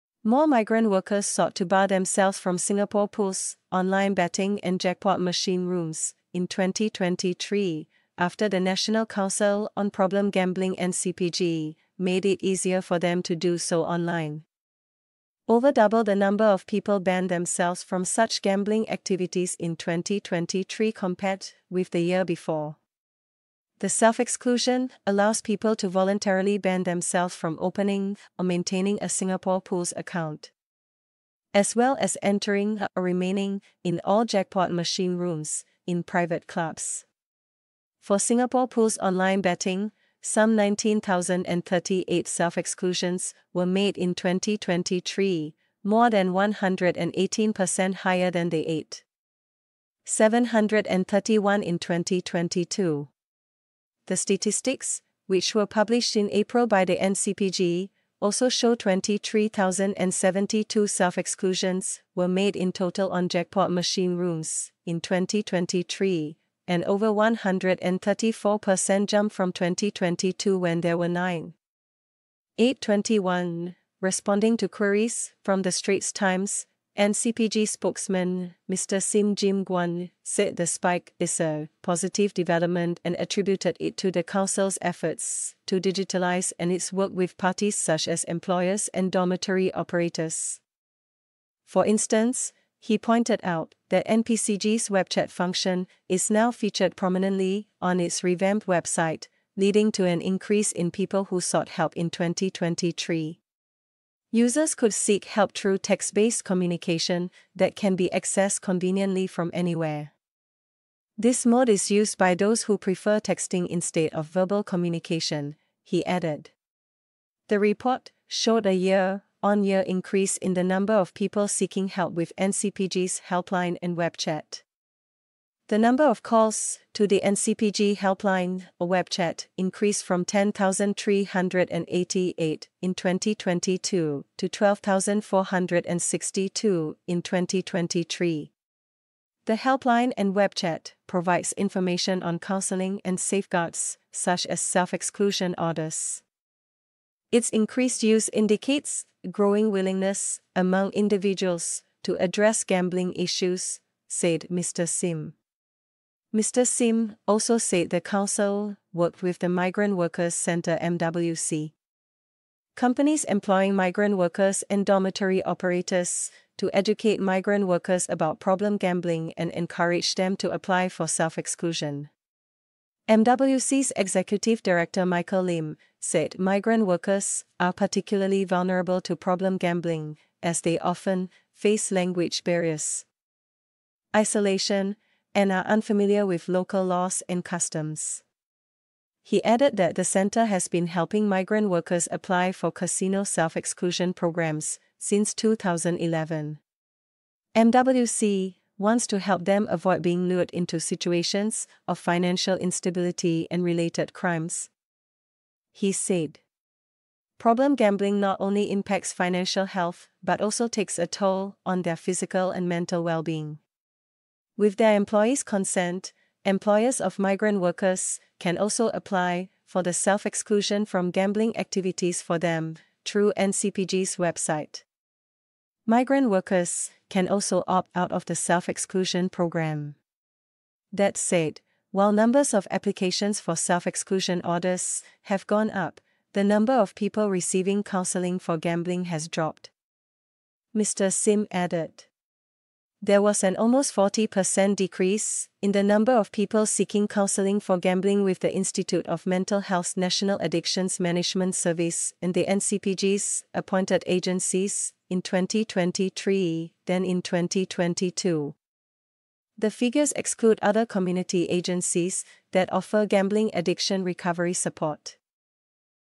More migrant workers excluded themselves from Singapore Pools online betting, jackpot rooms in 2023
💗📌 Singapore